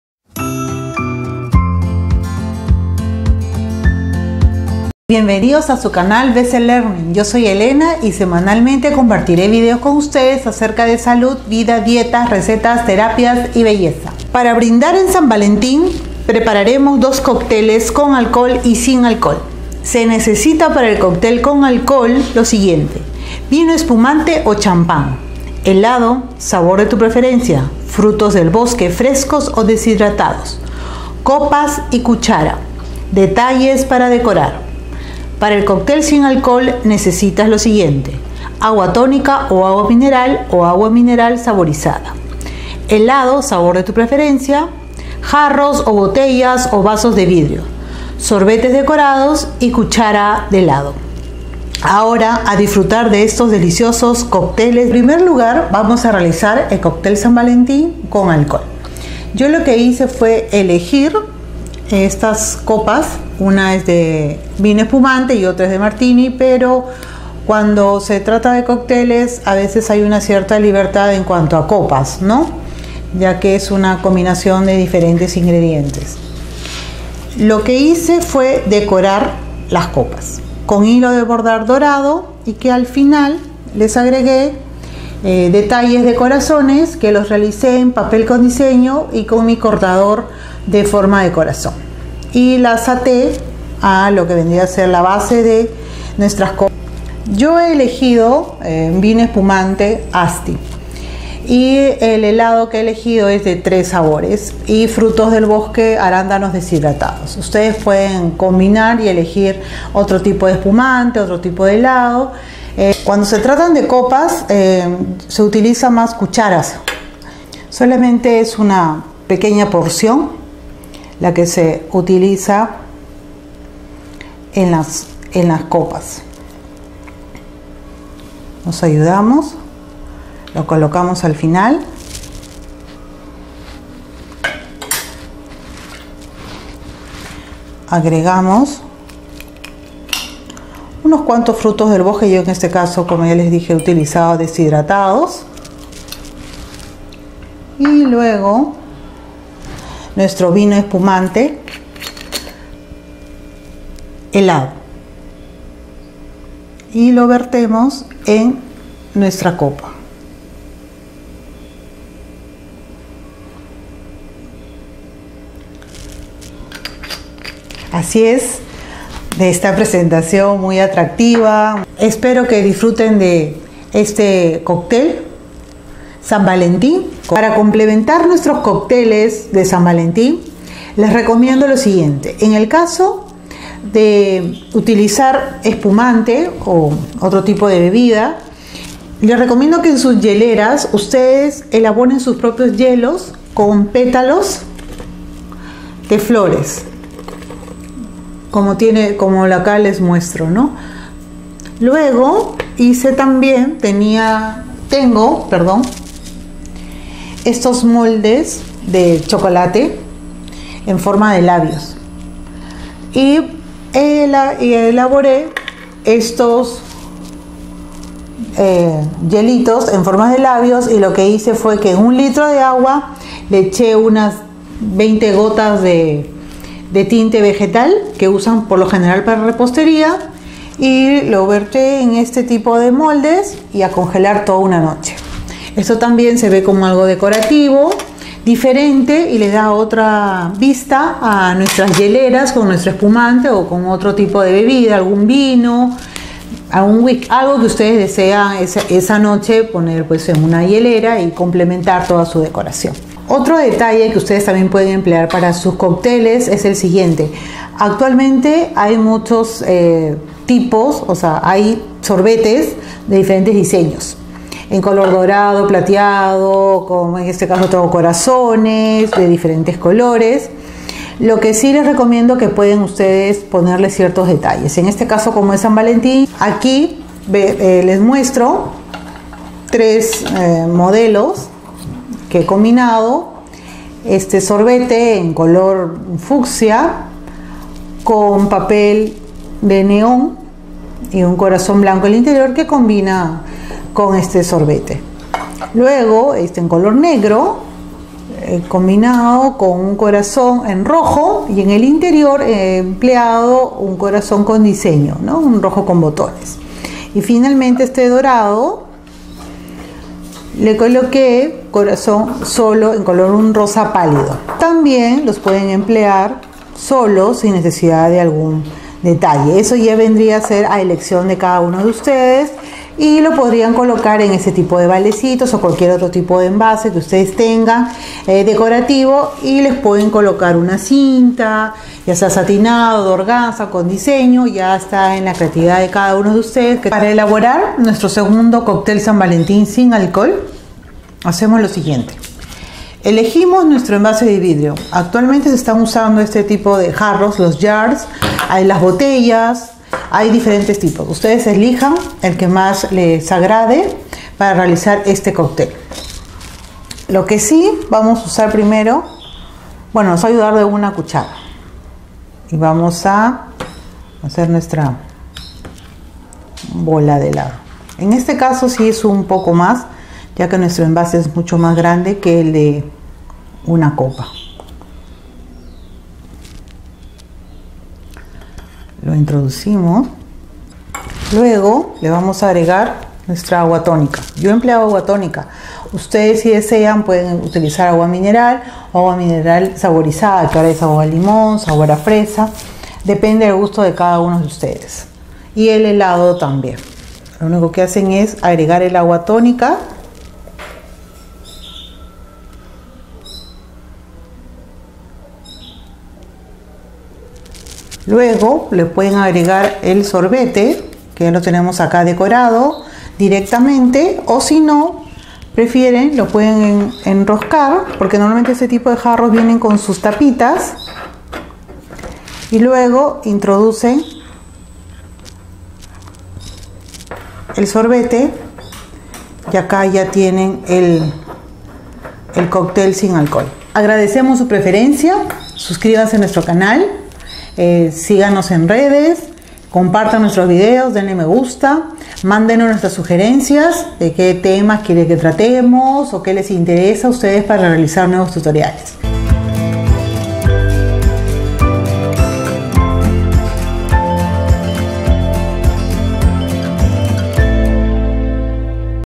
💗 Recetas fáciles 💘 Bebidas para San Valentín
@bclearninglive Brinda al amor y a la amistad con deliciosos cócteles con alcohol y sin alcohol. ¡Ahora a disfrutarlos!

🍒Temporada 1(2018): DIY • Valentine's Day

Exitos & Bendiciones
BC-Learning